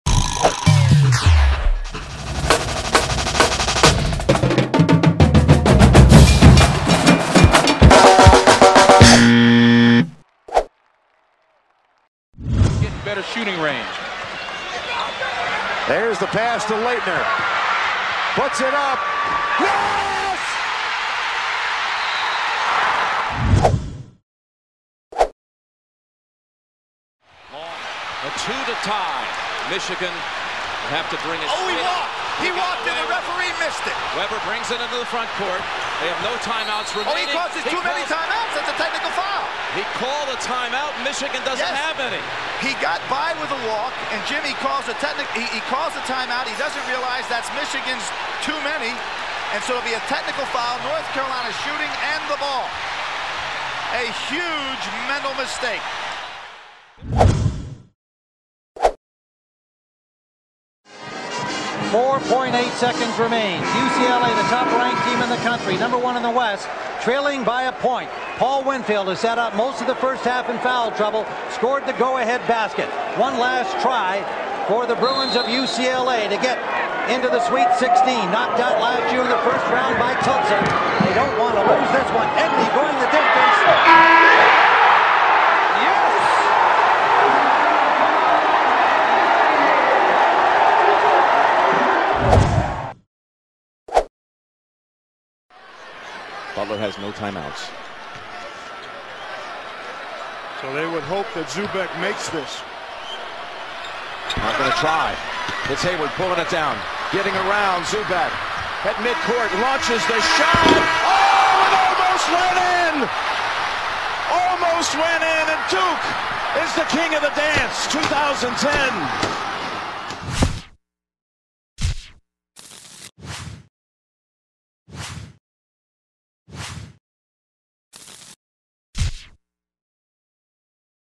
Greatest March Madness Moments Lego-ized
Here you go. You can now watch the greatest moments in the history of the NCAA basketball tournament. Re-created. Frame by frame. With Legos.

Don't say we never gave you anything.